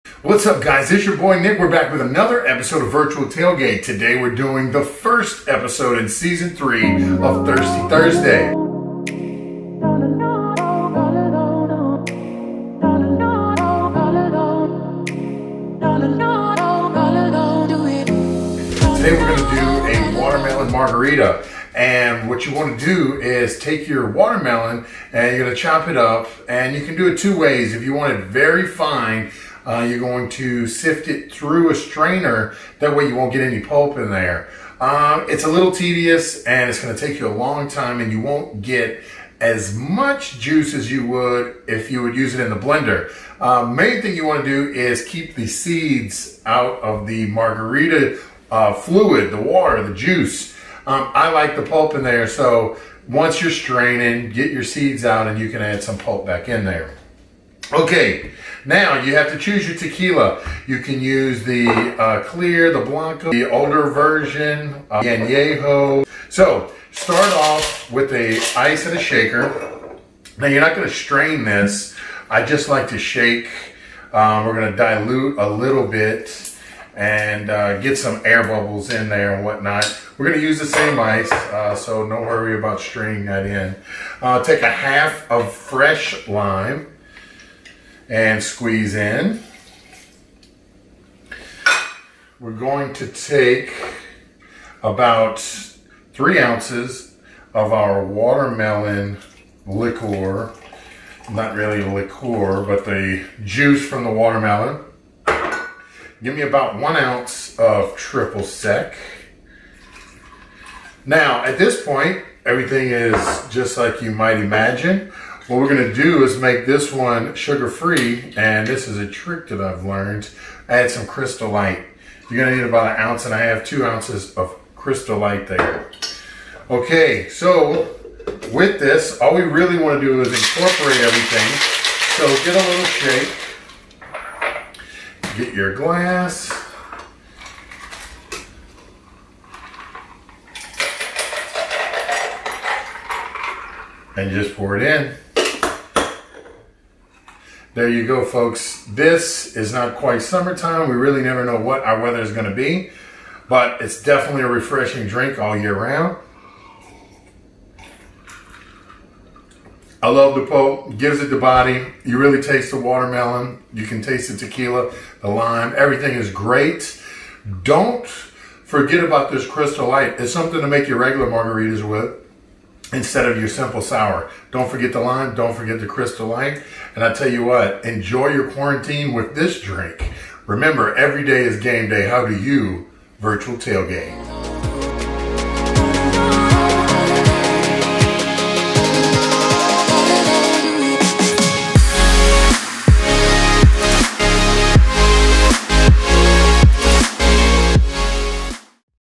Low Sugar Watermelon Margarita!
Ballerstatus, refreshing watermelon margarita to help you get through the quarantine!